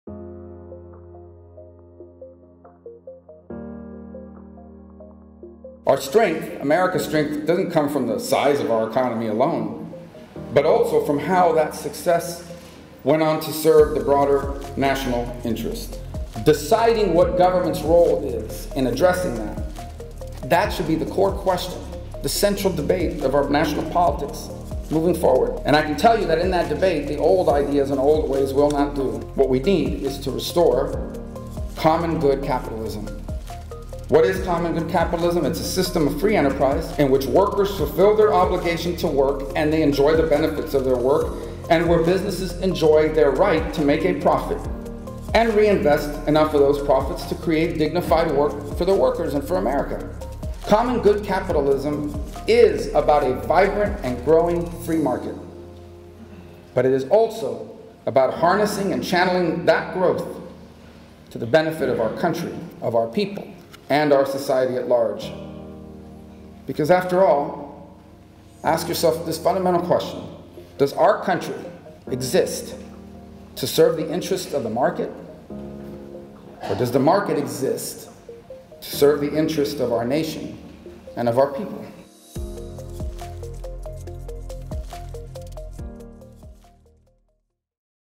Sen. Marco Rubio's "Common-Good Capitalism and the Dignity of Work"
November 5, 2019 | Sen. Marco Rubio delivered a speech on "Common-Good Capitalism and the Dignity of Work" at The Catholic University of America.